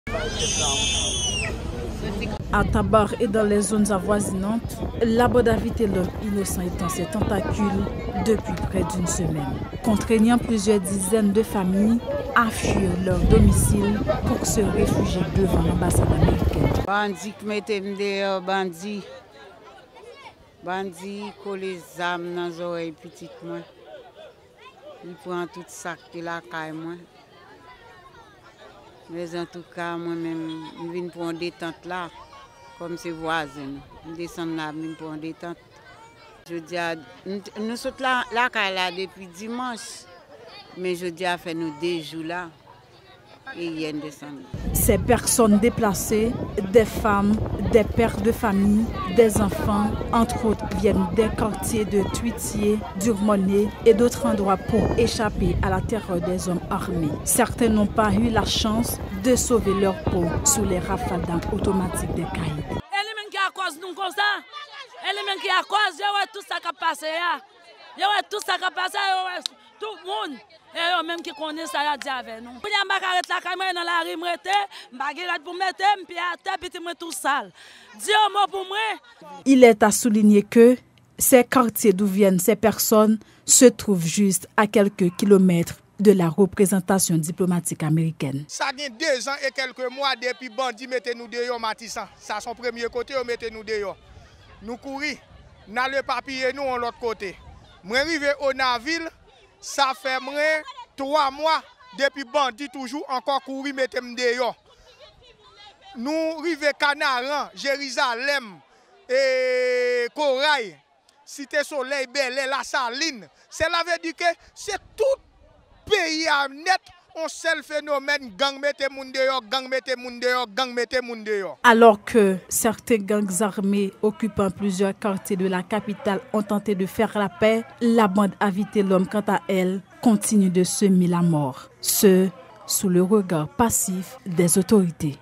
Aux abois,ces deplacés s'installent devant les locaux de L'ambassade des Etas-unis en Haïti.
À Tabarre,plusieurs dizaines de familles fuient leurs maisons pour échapper à la violence des bandits armés qui sèment la terreur dans cette commune.Aux abois,ces deplacés s'installent devant les locaux de L'ambassade des Etas-unis en Haïti.